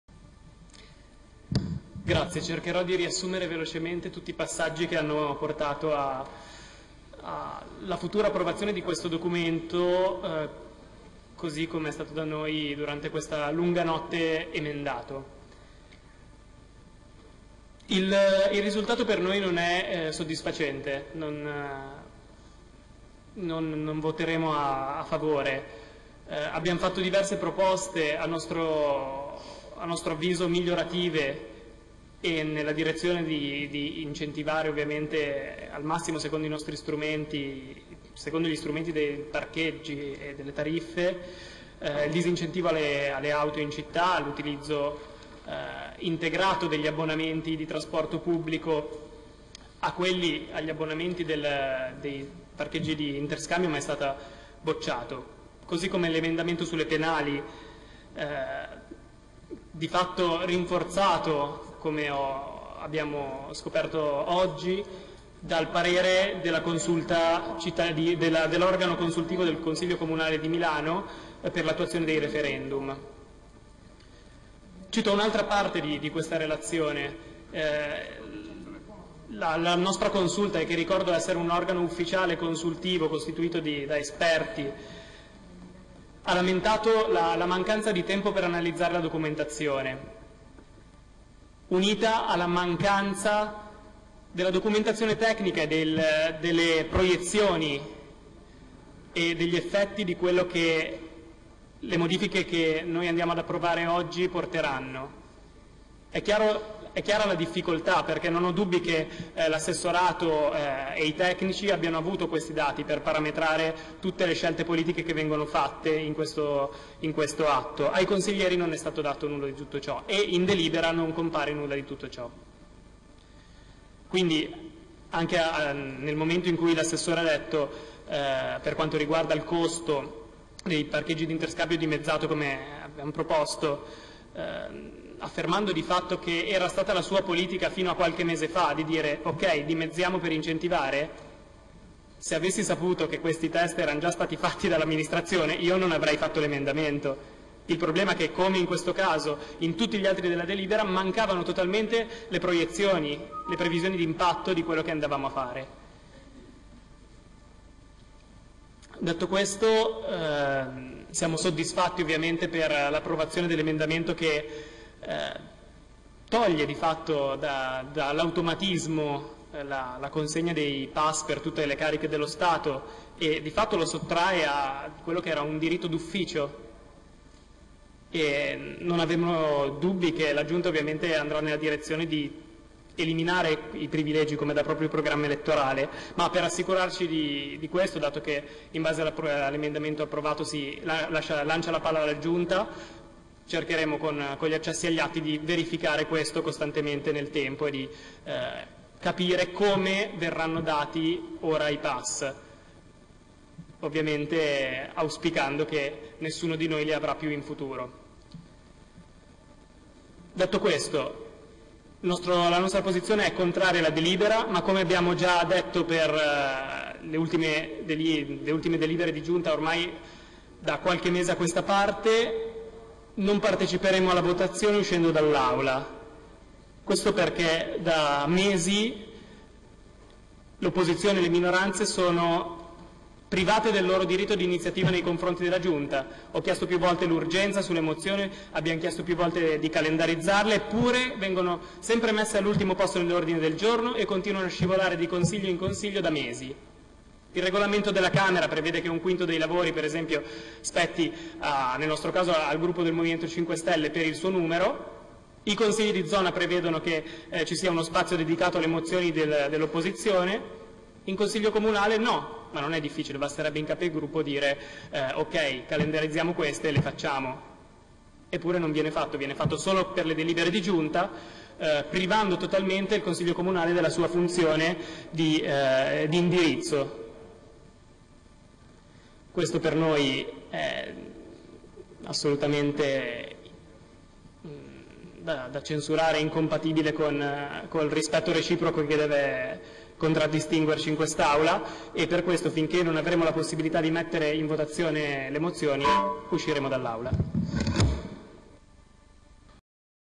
Milano 24 07 2013 Consiglio comunale Mattia Calise M5S dichiarazione di (non) voto tariffe sosta
anche alle 5e30 del mattino il MoVimento 5 Stelle Milano non perde occasione di richiamare la Giunta arancione per le modalità squilibrate di gestione del Consiglio